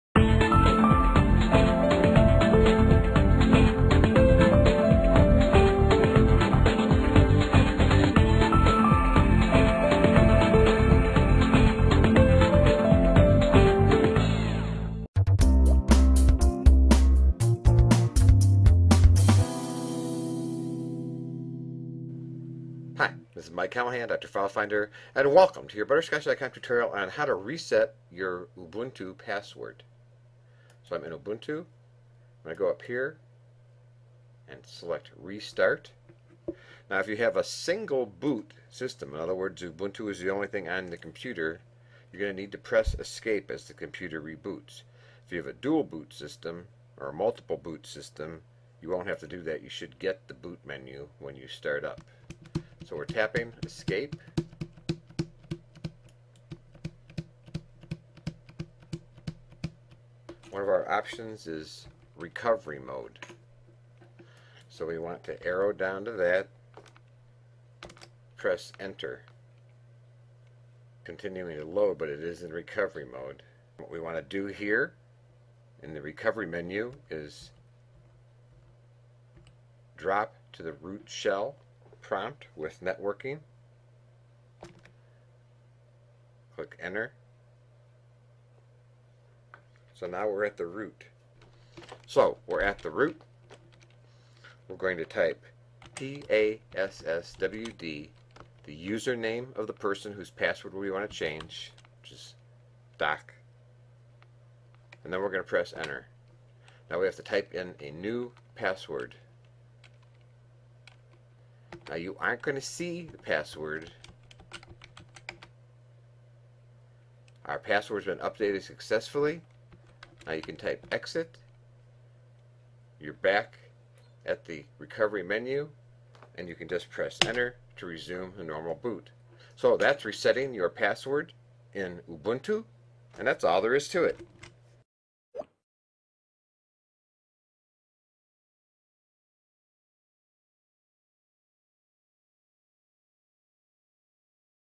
How to reset your password in Ubuntu
Ubuntu Linux is very secure. If you want to keep it secure you should use good passwords. A Internet tutorial by butterscotch.com.